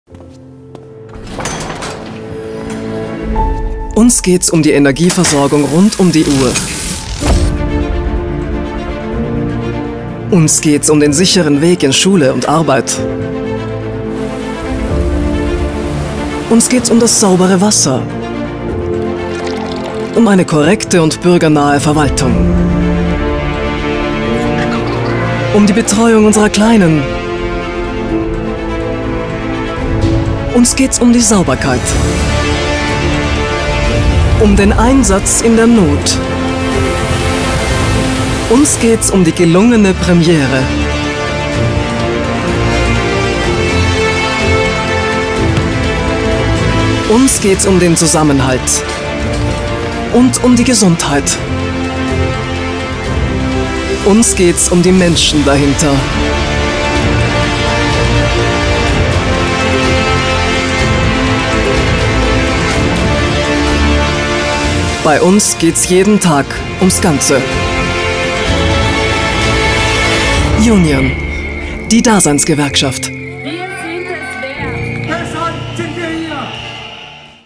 Younion Imagefilm "Ums Ganze" 2019 60s
Imagekampagne "Bei uns geht‘s jeden Tag ums Ganze", Daseinsgewerkschaft Younion, Österreich.

Filmproduktion: PPM 2.0, Producer Rudolf Janisch, Regisseur und Kameramann Marcel Schnelliger